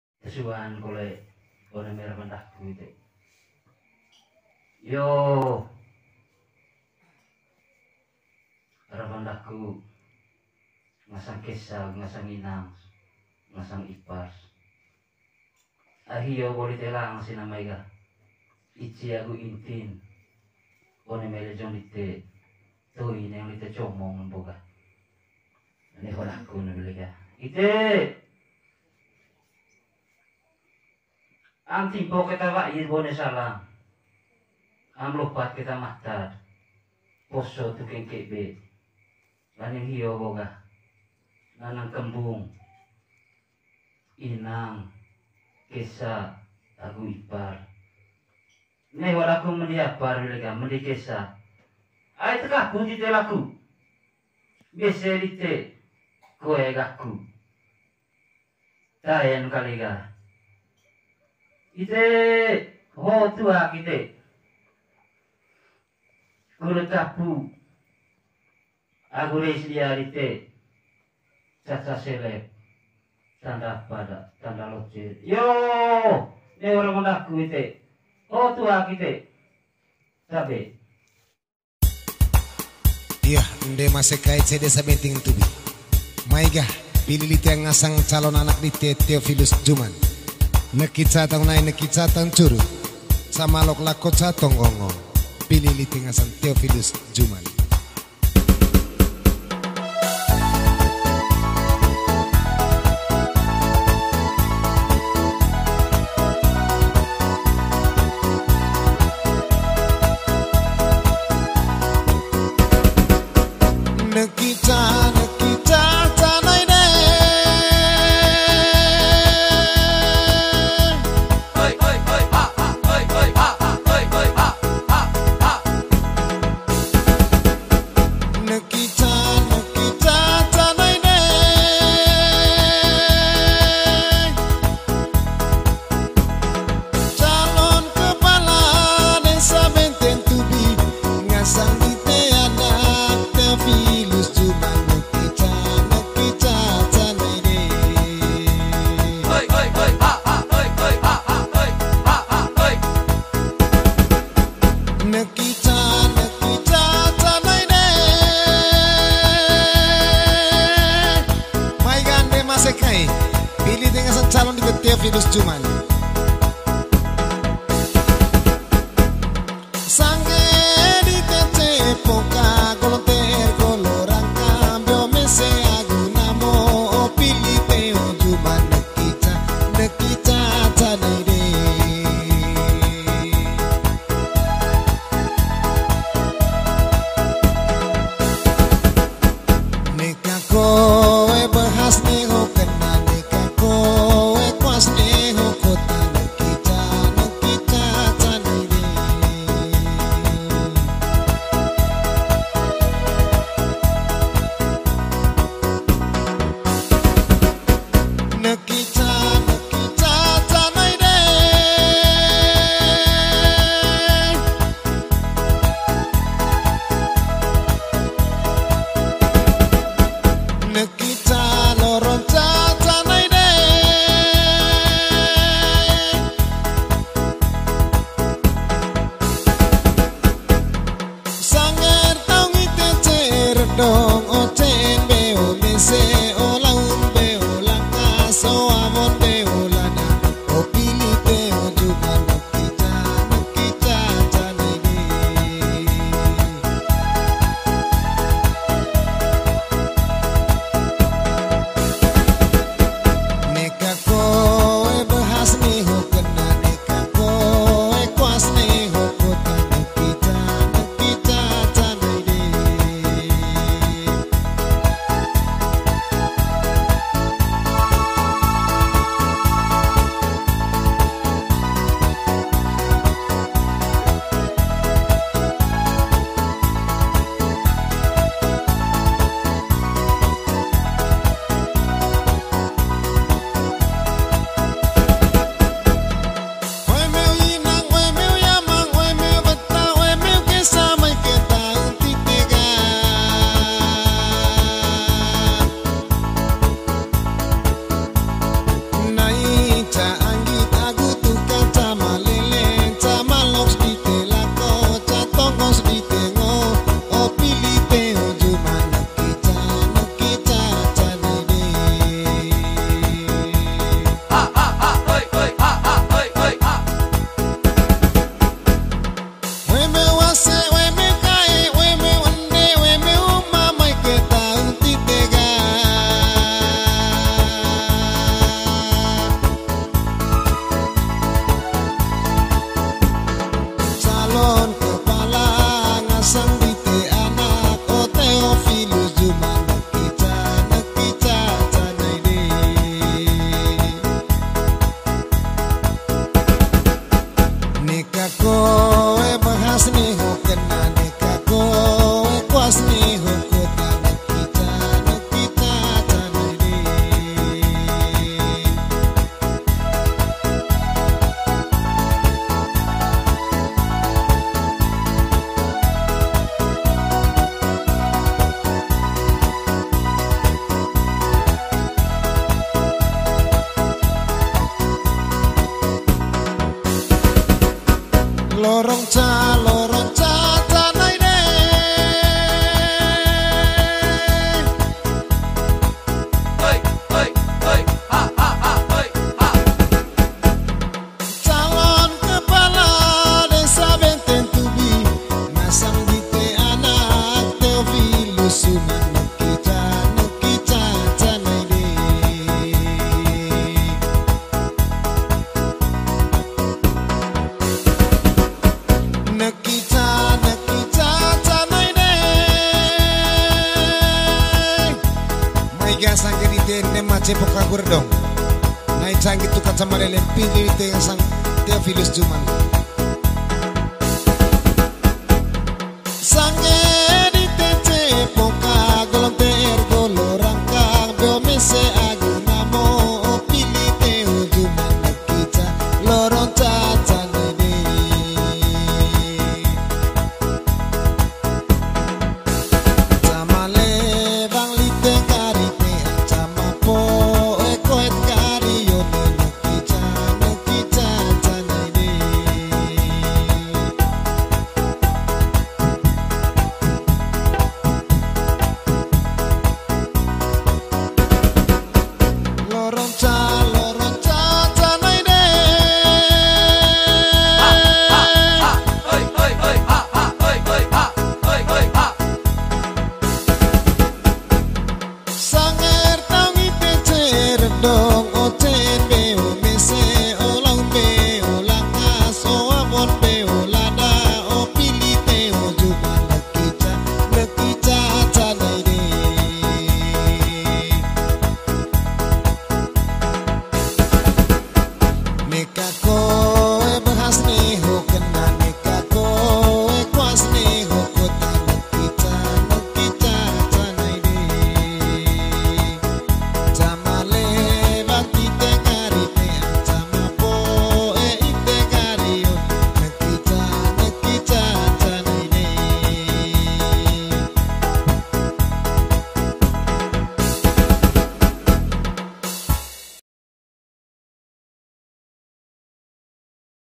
Acara Wuat Wa,i Teo Juman, Cakades Benteng Tubi || Antusias Masyarakat Mendukung Gerakan Perubahan.
Memohon restu para leluhur agar perjuangan dalam hajatan pilkades Benteng Tubi senantiasa Kompak dan meraih kemenangan.